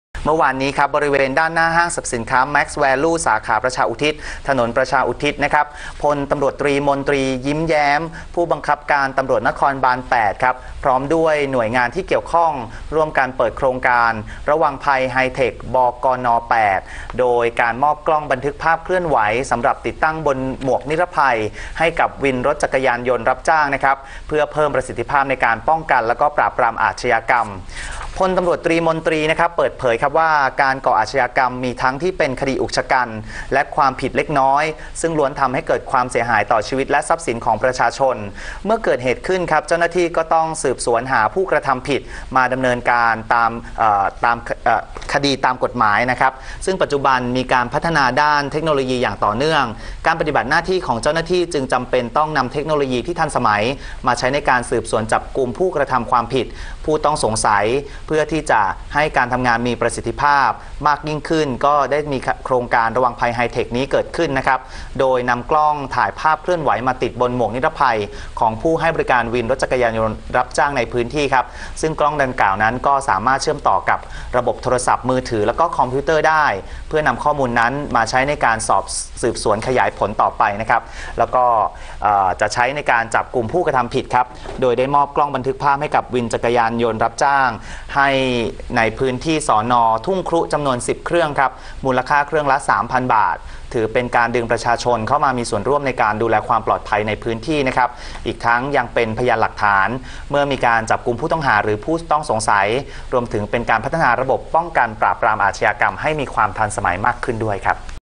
ผบก.น.8 ผุดไอเดียติดกล้อง จยย.รับจ้าง
2016/03/19 ผบก.น.8 ผุดไอเดียติดกล้อง จยย.รับจ้าง